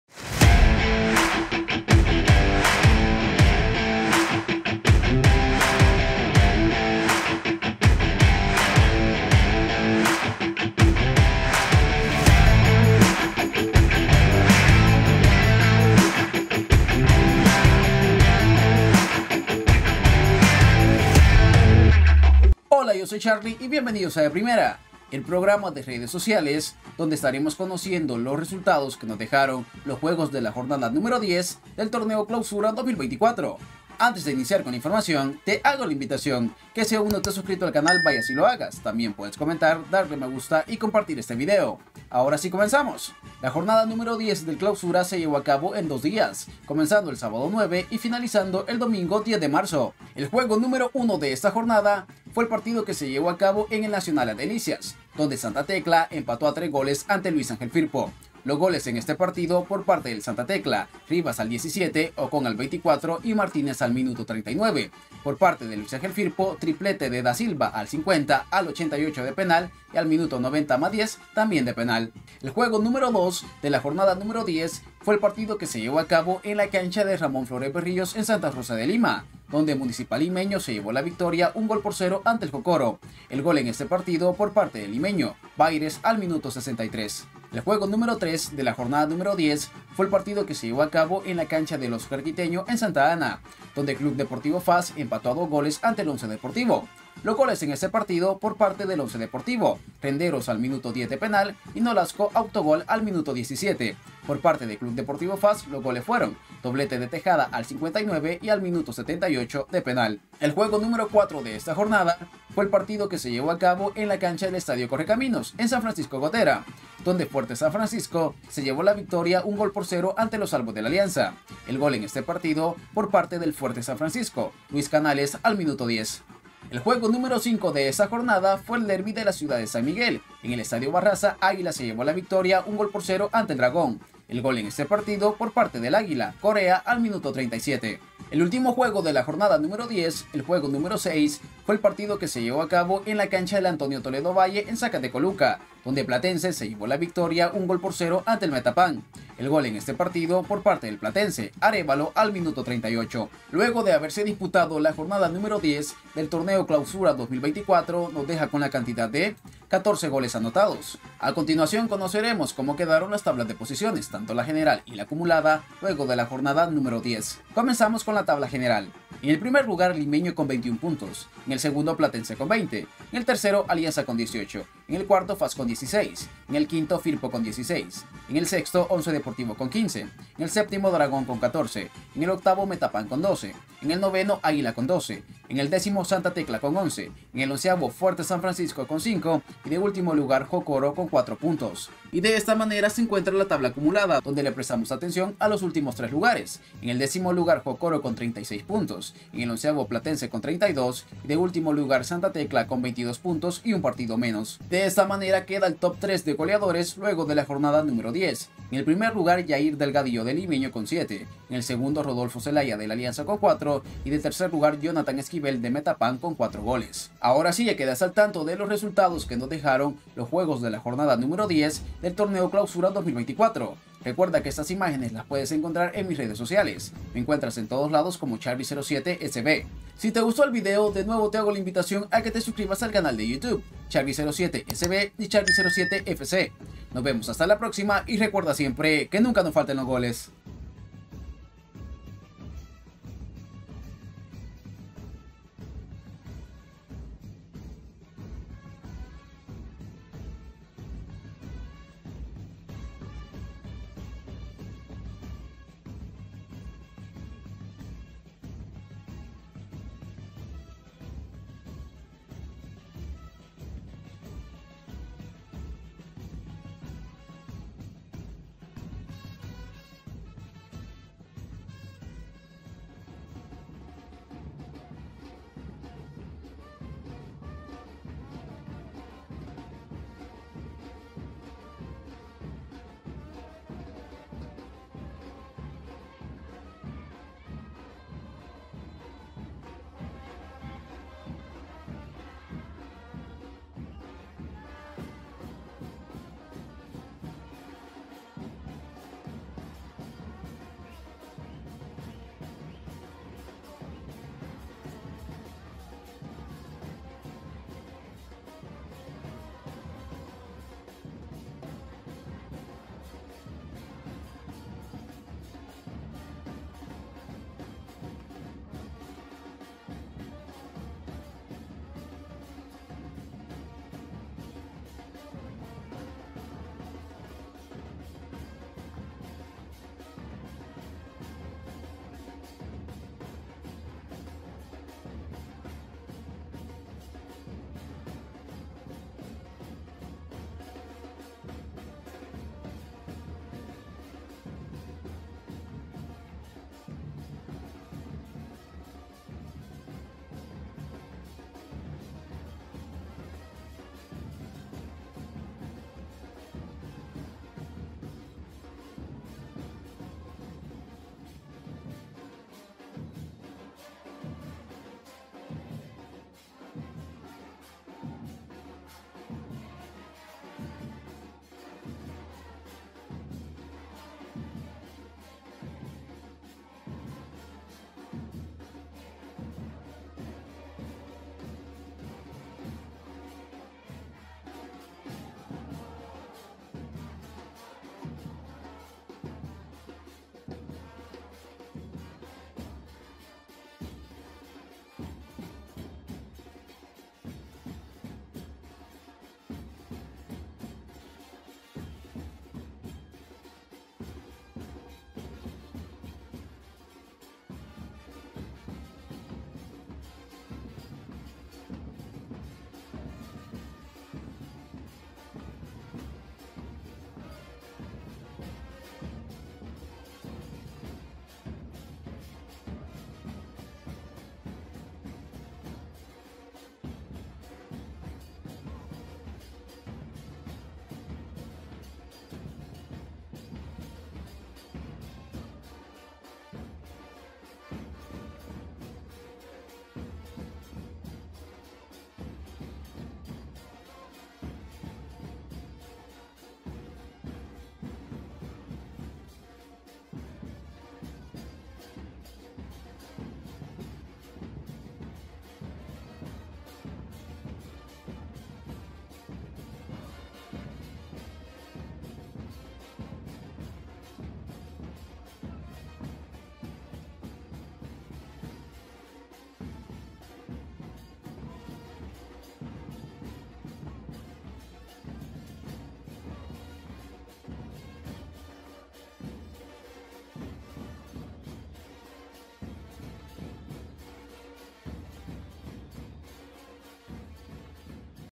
⚽ RESULTADOS DE LA JORNADA 10 DEL CLAUSURA 2024 🏆 ! EL SALVADOR!
De Primera con los Resultados de los juegos de la Jornada 10 del Clausura2024 🏆 Tablas de posiciones y Mas...

Donación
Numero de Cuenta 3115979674
AMILCAR MATOZO